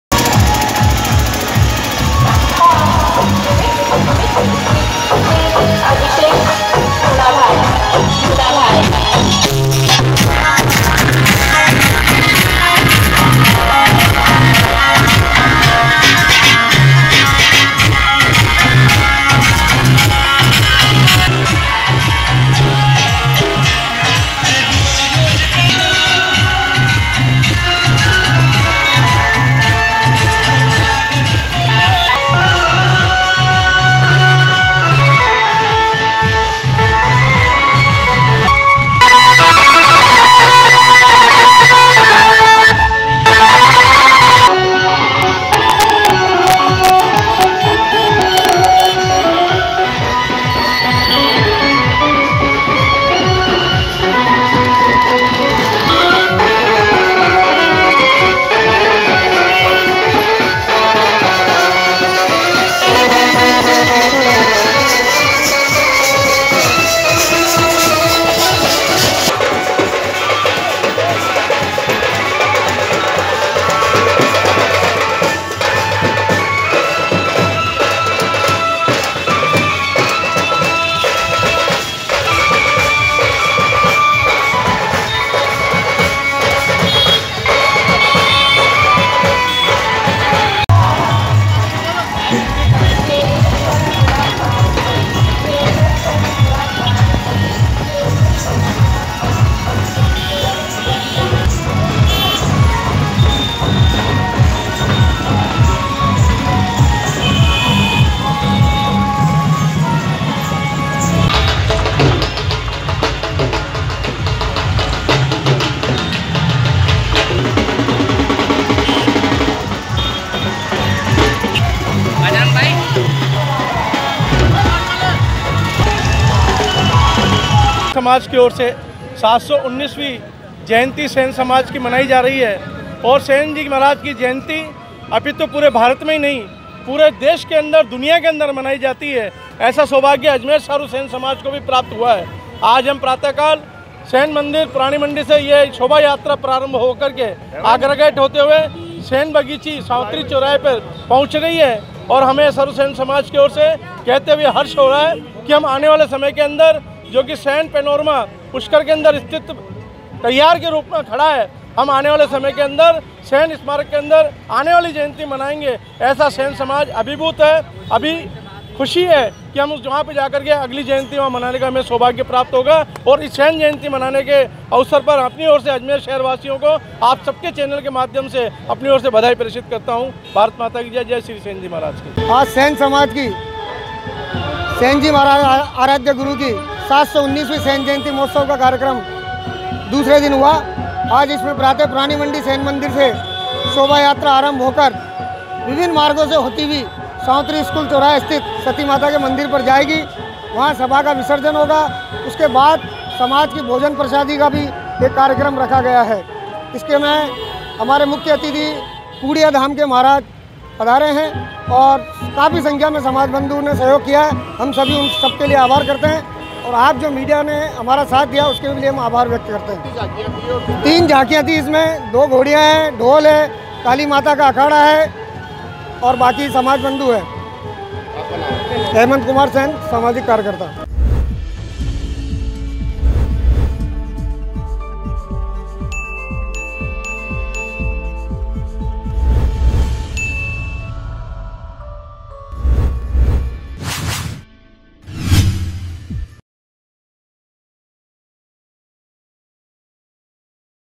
AJMER NEWS | समस्त सेन समाज ने हर्षोल्लास से मनाई सेनजी महाराज की 719वीं जयंती | MTTV INDIA
समस्त सेन समाज ने हर्षोल्लास से मनाई सेनजी महाराज की 719वीं जयंती
शहर भर में निकाली कलश शोभायात्रा
 शहर के मुख्य मार्गों पर शोभयात्रा का
 विभिन्न सामाजिक संगठनों ने पुष्प वर्षा कर किया स्वागत
 राधाकृष्ण और सेनजी महाराज की झांकियां रही आकर्षण का केंद्र